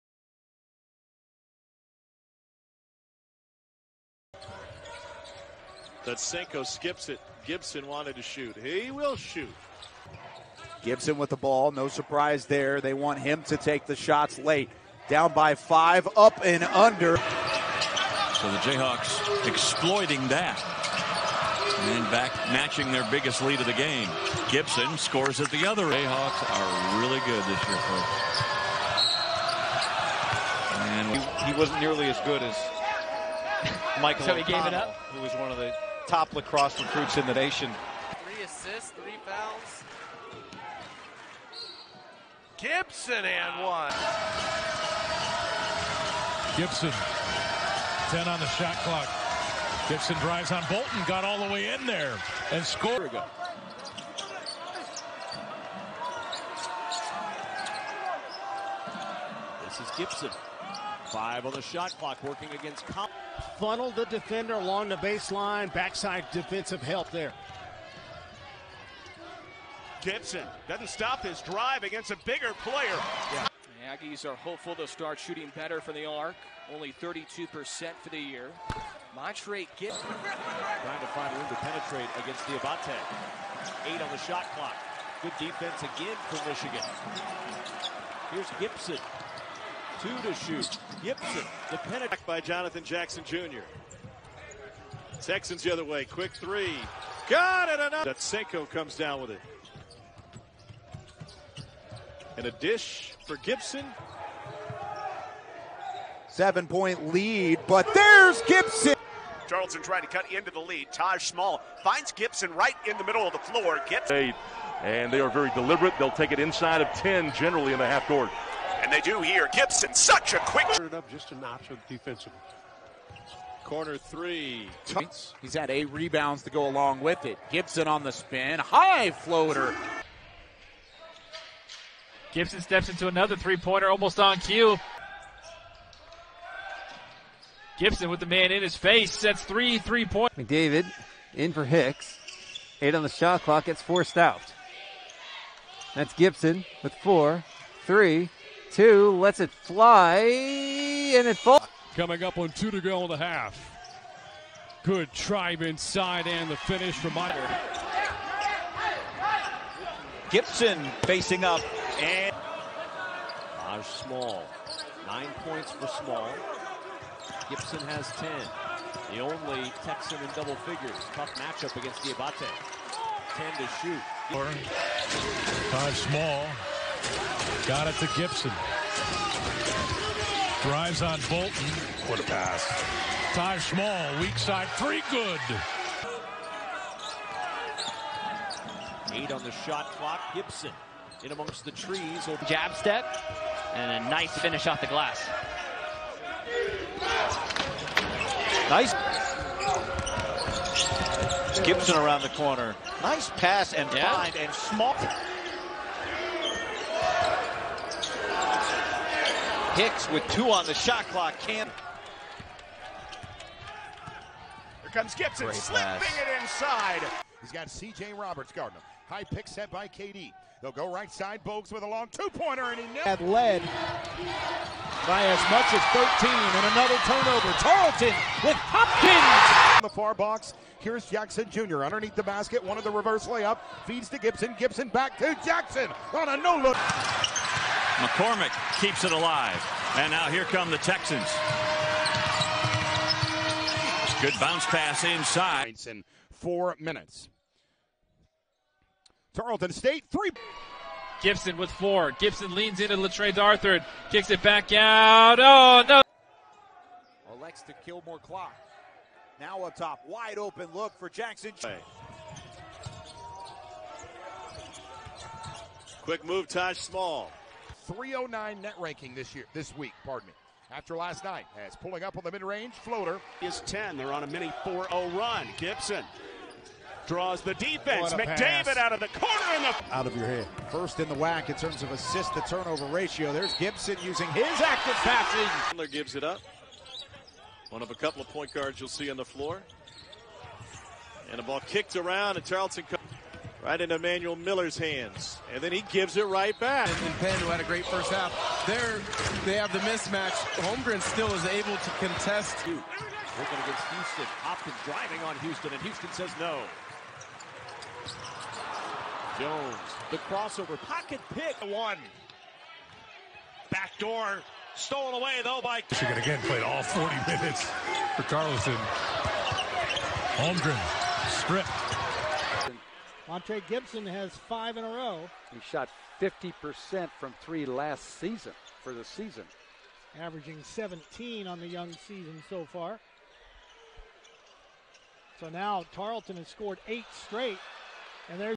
Montre Gipson Tarleton State 2020-2022 Highlight Tape
Montre Gipson
5'11
PG (American Passport)
College: Tarleton State University (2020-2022)
Note: Montre wore #1 in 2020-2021 season and #11 in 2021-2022 season
Stats:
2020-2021: 15.3PPG –3.84 RPG–3APG-1.42STL /54.9% FG% –50% 3PT%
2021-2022:14.9PPG –4.74 RPG–3.13APG-1.39STL /42.9% FG% –31% 3PT%